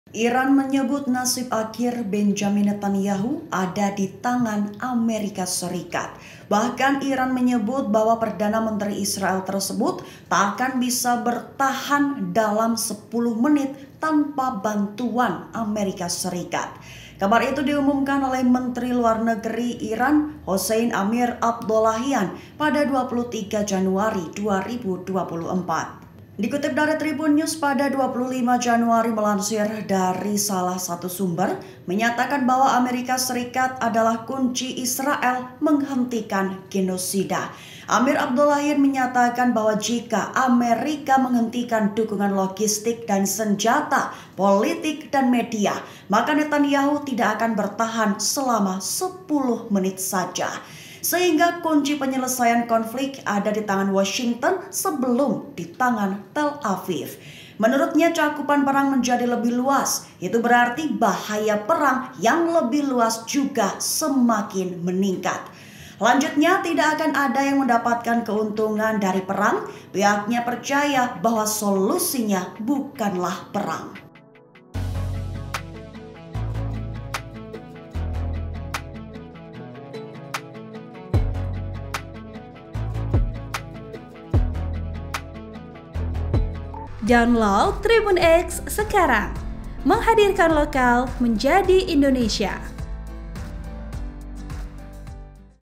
Iran Sebut 'Nasib' Akhir Netanyahu: Tidak akan Bisa Bertahan 10 Menit Tanpa Dukungan Amerika Serikat
TRIBUN-VIDEO.COM - Iran menyebut nasib akhir Benjamin Netanyahu ada di tangan Amerika Serikat.

Bahkan, Iran menyebut bahwa Perdana Menteri Israel tersebut tak akan bisa bertahan dalam 10 menit tanpa bantuan AS.

Kabar itu diumumkan oleh Menteri Luar Negeri Iran Hossein Amir-Abdollahian pada (23/1/2024).

Dikutip dari Tribunnews pada (25/1) melansir dari ABC News menyatakan bahwa AS adalah kunci Israel hentikan 'genosida'.

Amir-Abdollahian menyatakan bahwa jika AS menghentikan dukungan logistik dan senjata, politik dan media, maka Netanyahu tidak akan bertahan selama 10 menit.

"Jika AS hari ini menghentikan dukungannya -- dukungan logistik dan senjata, politik dan media -- terhadap perang genosida yang dilancarkan Israel, maka saya dapat meyakinkan Anda bahwa [Perdana Menteri Israel Benjamin] Netanyahu tidak akan bertahan selama 10 menit," ujar diplomat top Iran tersebut mengatakan kepada Kepala Koresponden Urusan Global ABC News Martha Raddatz.

Sehingga, kunci penyelesaian konflik ada di tangan Washington sebelum di tangan Tel Aviv.

“Jadi kunci penyelesaian masalah ini ada di Washington sebelum di Tel Aviv,” katanya dari New York di sela-sela pertemuan Dewan Keamanan PBB mengenai Asia Barat.

Menurutnya, cakupan perang menjadi lebih luas, itu berarti bahaya perang yang lebih luas juga semakin meningkat.

“Cakupan perang menjadi lebih luas. Ini berarti bahaya perang yang lebih luas di kawasan ini semakin meningkat,” tambah Amir-Abdollahian, seraya menyalahkan AS dan Israel atas meningkatnya ketegangan di kawasan.

Lanjutnya, tidak akan ada yang mendapatkan keuntungan dari perang, pihaknya percaya bahwa solusinya bukanlah perang.

“Tidak ada yang akan mendapatkan keuntungan dari perang apa pun. Kami percaya bahwa solusinya bukanlah perang,” tegas pejabat Iran.

(Tribun-Video.com)

Artikel ini telah tayang di Tribunnews.com dengan judul Jika AS Hentikan Dukungan Senjata ke Israel Maka Ini Akan Terjadi pada Netanyahu kata Menlu Iran

Penulis: Muhammad Barir

Host: Yessy Wienata
VP: Ananda Bayu S